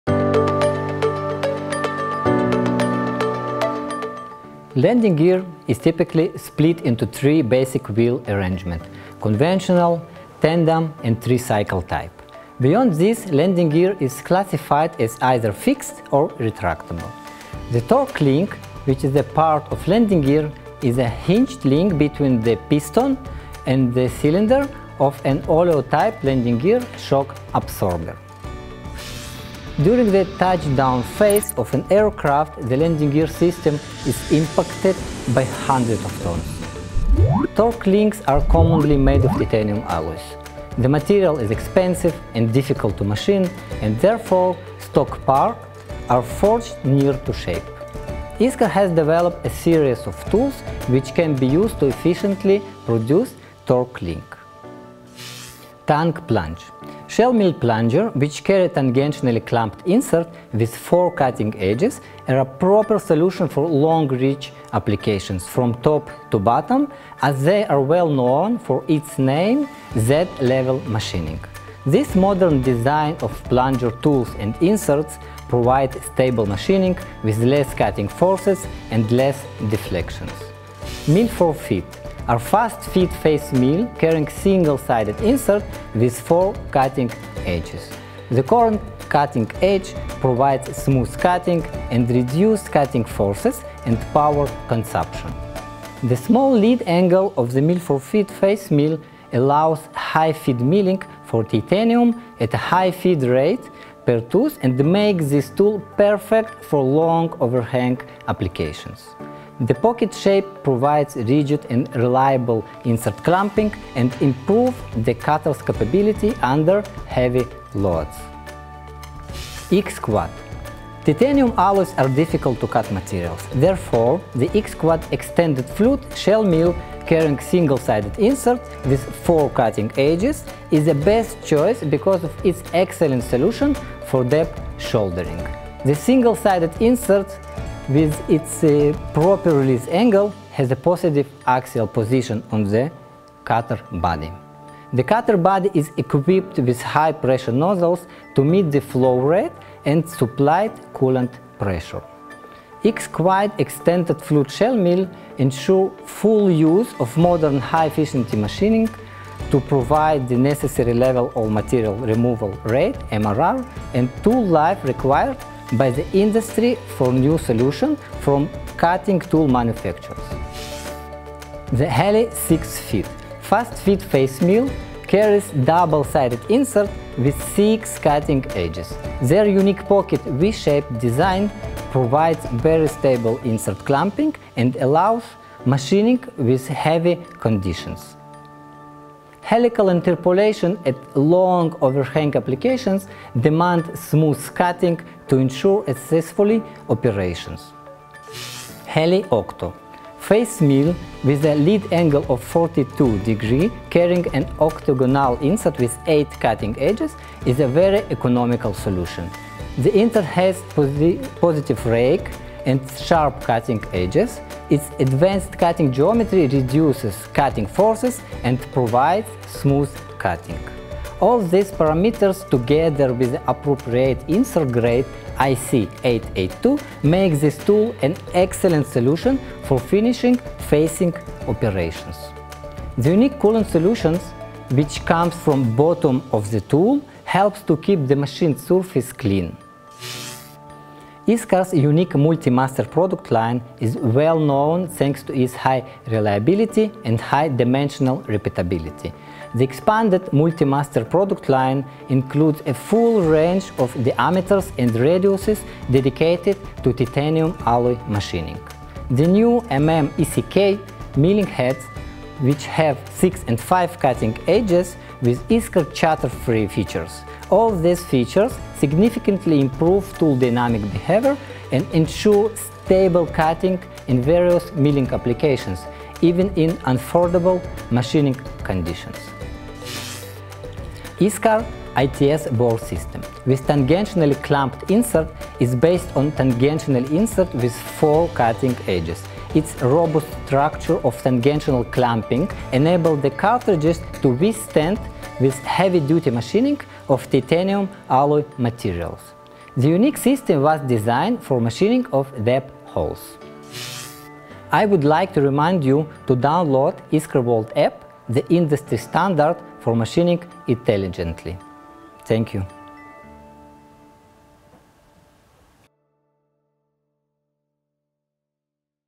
ISCAR INDUSTRY TALK -Torque Links [Aerospace]
Landing gear is typically split into three basic wheel arrangements: conventional, tandem and tricycle-type. Beyond this, landing gear is classified as either fixed or retractable.
The torque link, which is a part of the landing gear, is a hinged link between the piston and the cylinder of an oleo-type landing gear shock absorber. The torque link allows the piston to move freely in and out of the landing gear cylinder - but prevents it from rotating.
During the touchdown phase of an aircraft, the landing gear system is impacted by hundreds of tons.
Torque links are commonly made of titanium alloys. The material is expensive and difficult to machine, and therefore stock parts are forged near to shape.
ISCAR has developed a series of tools which can be used to efficiently produce torque links:

TANGPLUNGE Shell Mill Plungers, which carry tangentially clamped Inserts with 4 Cutting Edges are a proper solution for long reach applications from top to bottom as they are well known for its name “Z” level machining. This modern design of plunger tools and inserts provide stable machining with less cutting force and less deflection.

MILL4FEED are Fast Feed Face Mills Carrying Single-Sided Inserts with 4 Cutting Edges. The correct cutting edge provides smooth cutting and reduced cutting forces and power consumption. The small lead angle of the Mill4Feed face mill allows high feed milling for titanium at a high feed rate per tooth and makes this tool perfect for long overhang applications.  The pocket shape provides rigid and reliable insert clamping and improves the cutter’s capability under heavy loads.

Titanium alloys are difficult to cut materials, therefore, the XQuad Extended flute shell mills carrying single-sided Inserts with 4 cutting edges is the best choice because of its excellent solution for depth shouldering.

The Single sided insert with its proper release angle has a positive axial position on the cutter body. The cutter body is equipped with high pressure nozzles to meet the flow rate and supplied coolant pressure.

XQuad’s extended flute shell mills ensure full use of modern high-efficiency machines to provide the necessary level of metal removal rate (MRR) and tool life required by the industry for new solutions from cutting tool manufacturers.

The HELI6FEED Fast feed face mill carries double-sided inserts with 6 cutting edges. Their unique pocket “V” shaped design provides very stable insert clamping and allows machining with heave conditions. Helical interpolation at long overhang applications demands smooth cutting to assure a successful operation.

HELIOCTO Face mills with a lead angle of 42°, carrying an octagonal insert with 8 cutting edges is a very economical solution. The insert has positive rake and sharp cutting edges. It’s advanced cutting geometry reduces cutting forces and provides smooth cutting. All these parameters together with the appropriate insert grade IC882, make this tool an excellent solution for finish facing operations. The unique coolant solution, which comes from bottom of the tool, helps to keep the machined surface clean.

ISCAR’s unique Multi-Master product line is well-known thanks to its high reliability and high dimensional repeatability. The expanded MULTI-MASTER product line includes a full range of diameters and radiuses dedicated to titanium alloys machining.

The new MM ECK… milling heads have 6 and 5 cutting edges with ISCAR CHATTERFREE features.

All these features significantly improve tool dynamic behavior and ensures stable cutting in various milling applications, even in unfavorable machining conditions.

ISCAR’s ITSBORE system, with tangentially clamped Inserts is based on tangential inserts with 4 cutting edges. Its robust structure of tangential clamping enables the cartridge to withstand heavy duty machining of titanium allows materials. This unique system was designed for machining deep holes.

We would like to remind you to download the ISCAR World app which features the revolutionary NEO-ITA for achieving optimal machining results.